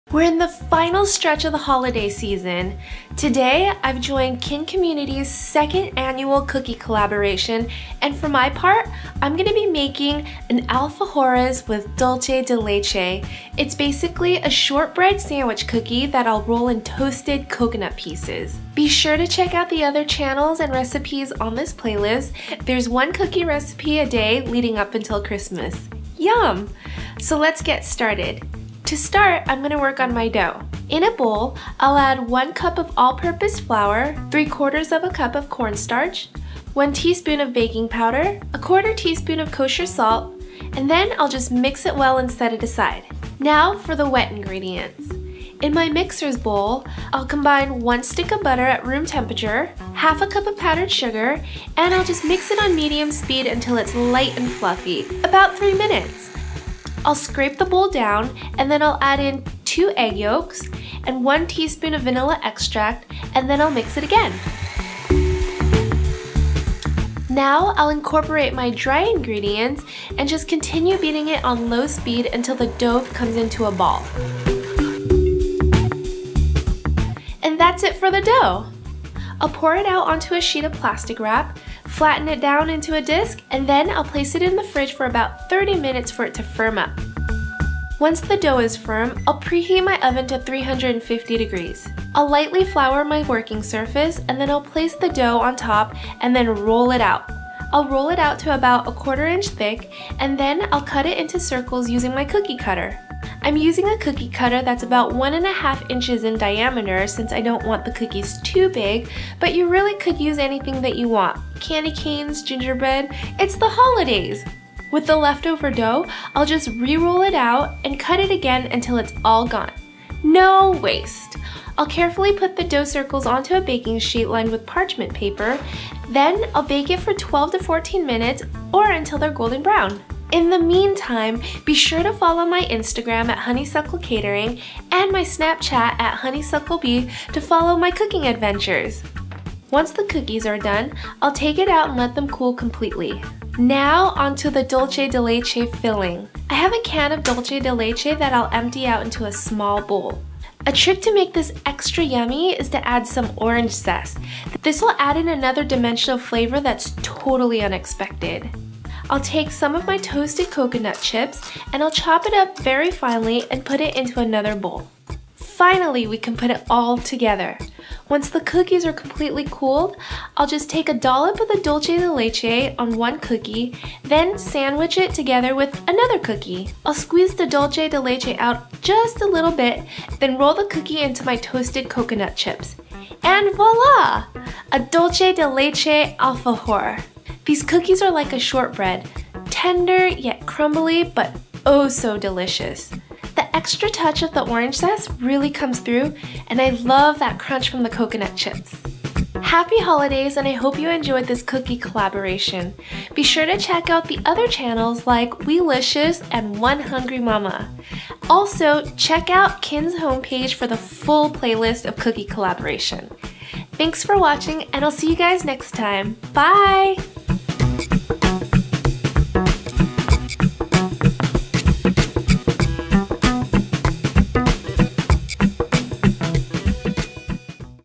Alfajores Dulce de Leche - Kin's Cookie Collab
Bakery vs Homemade MATCHA MOCHI CAKE Alfajores de Dulce de Leche! YUM

We’re in the final stretch of the Holiday season! Today I’ve joined Kin Community’s 2nd annual Christmas cookie collaboration and for my part, I’m going to be making an Alfajores with Dulce de Leche. It’s basically a shortbread sandwich cookie that I’ll roll in toasted coconut pieces. Be sure to check out the other channels and recipes on this playlist, there’s one cookie recipe a day leading up until Christmas! Yum! So let’s get started. To start I’m going to work on my dough.

Alfajores Dulce De Leche Recipe:

In a bowl, i’ll add

1 cup of all purpose flour,
¾ cup of cornstarch,
1 tsp of baking powder,
¼ tsp of kosher salt,

and then I’ll just mix it well and set it aside.

Now for the wet ingredients. In my mixer’s bowl, i’ll combine

1 stick (1/2 cup) of butter at room temperature,
½ cup powdered sugar,

and I’ll just mix it on medium speed until it’s light and fluffy, about 3 minutes. I’ll scrape the bowl down, and then I’ll add in

2 egg yolks and
1 tsp of vanilla extract,

and then I’ll mix it again. Now, I’ll incorporate my dry ingredients and just continue beating it on low speed just until the dough comes into a ball. And that’s it for the dough!

I’ll pour it out onto a sheet of plastic wrap, flatten it down into a disc, and then I’ll place it in the fridge for about 30 minutes for it to firm up.

Once the dough is firm I’ll preheat my oven to 350 degrees F. I’ll lightly flour my working surface and then I’ll place the dough on top and roll it out. I’ll roll it out to about ¼ inch thick and then cut it into circles using my cookie cutter. I’m using a cookie cutter that's about 1 ½ inches in diameter since I don’t want the cookies to too big, but you really can use anything that you want: candy canes, gingerbread, it’s the holidays!

With the leftover dough I’ll just re-roll it out, cut it again until it’s all gone. No waste.

I’ll carefully put the dough circles onto a baking sheet lined with parchment paper, then I’ll bake it for 12-14 minutes or until they’re golden brown. Once the cookies are done, I’ll take it out and let them cool completely.

Now onto the dulce de leche filling. I have a can of dulce de leche that I’ll empty into a small bowl. A trick to make this extra yummy is to add some orange zest. This will add in another dimension of flavor that’s totally unexpected. I’ll take some of my toasted coconut chips and I’ll chop it up very finely and put into another bowl.

Finally we can put it all together! Once the cookies are completely cooled I’ll just take a dollop of the dulce de leche on one cookie, then sandwich it together with another cookie. I’ll squeeze the dulce de leche out just a little bit, then roll the cookie into my toasted coconut chips. And voila! A Dulce de Leche Alfajor. These cookies are like a shortbread - tender yet crumbly but oh so delicious. That extra touch of the orange zest really comes through and I love that crunch from the coconut chips.

Happy Holidays and I hope you enjoyed this cookie collaboration! Be sure to check other channels like Weelicious and One Hungry Mama. Also check out Kin’s home page for the full playlist of cookie collaboration. Byeeeeeee!